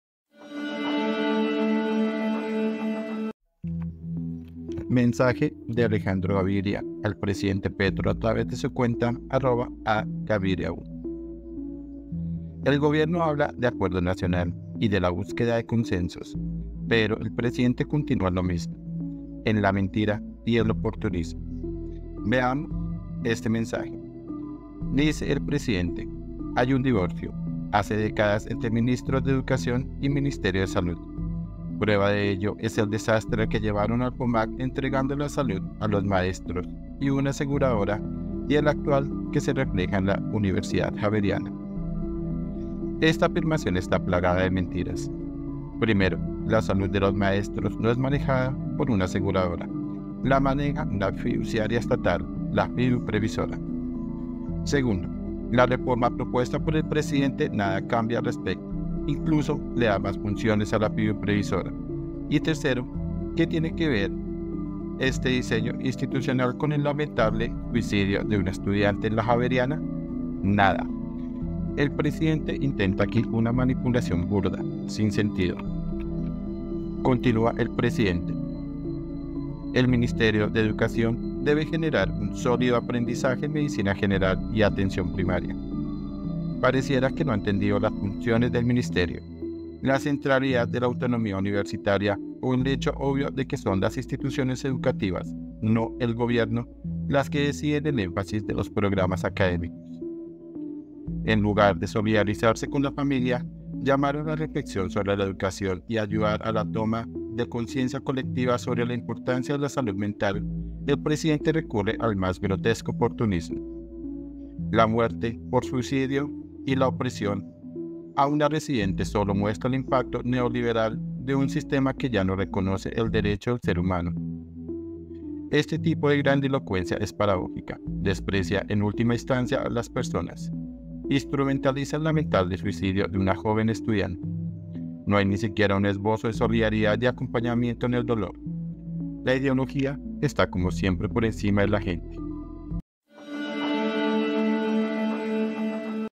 GROTESCO OPORTUNISMO 📢 ALEJANDRO GAVIRIA DESMIENTE AL PRESIDENTE PETRO
El gobierno habla de acuerdo nacional y de la búsqueda de consensos. Pero el presidente continua en lo mismo, en la mentira y el oportunismo. Veamos este mensaje.  Dice el presidente: “hay un divorcio […] hace décadas entre ministerio de educación y ministerio de salud. Prueba de ello es el desastre al que llevaron al Fomag entregando la salud de los maestros a una aseguradora y el actual que se refleja en la universidad Javeriana”.